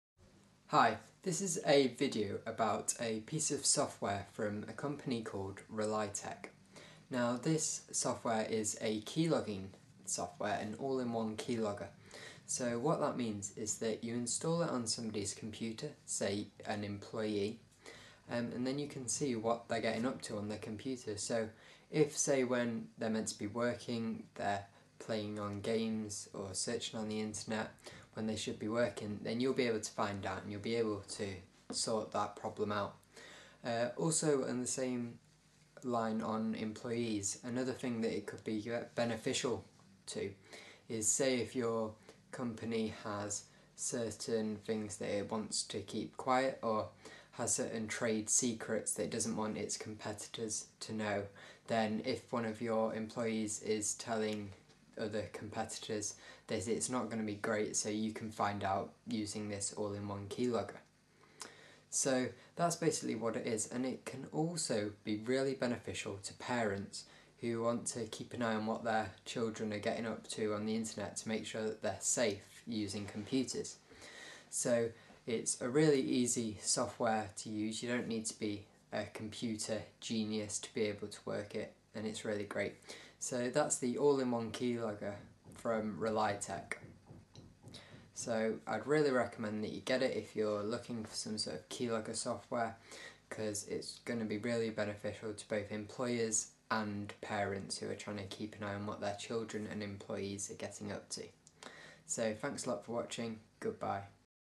User review for Relytec All In One Keylogger spy software
Keylogger review for Relytec Key logger software.
All In One Keylogger is an Invisible Keylogger, keystrokes logger, surveillance software that registers every activity on your PC to encrypted logs.

Key logger computer spy Platforms: Windows 2000, 2003, XP, Vista, Windows 7, Windows 8, Windows 10 Keylogger (32-bit and 64-bit)